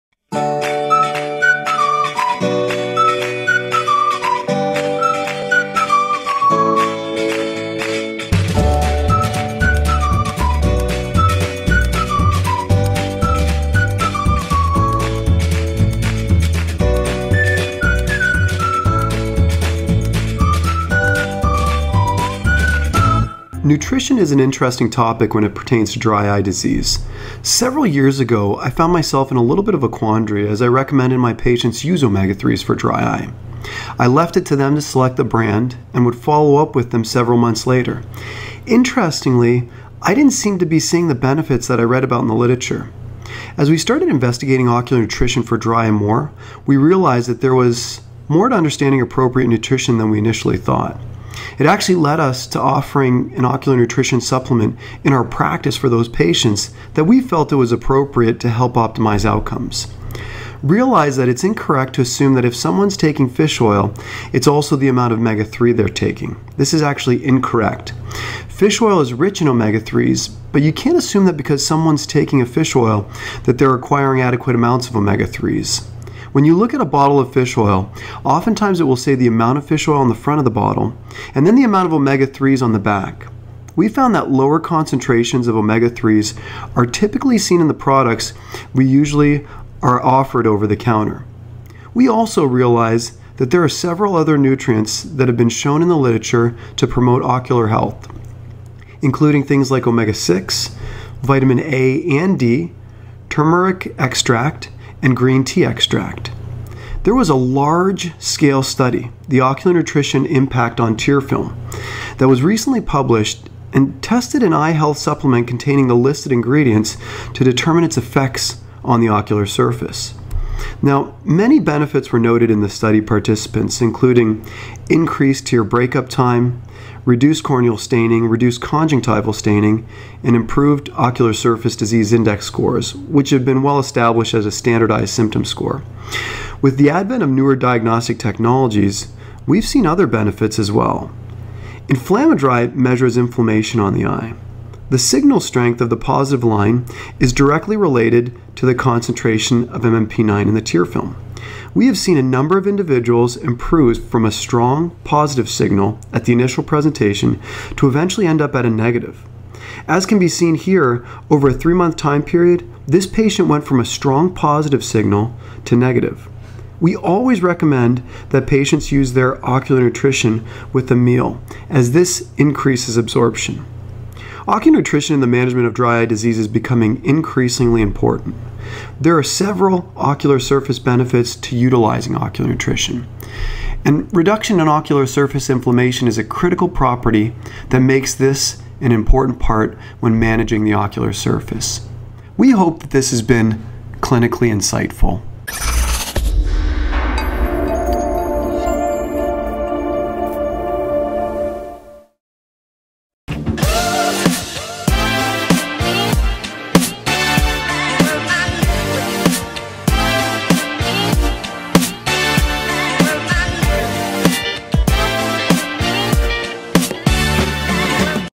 How Does Nutrition Impact Dry Eye Disease? - Episode 76
How does nutrition impact dry eye disease? In this video we discuss the ocular surface benefits to utilizing ocular nutrition.

Optometric Insights – bringing your success into focus and providing you with the tools to accomplish now what others would say is impossible.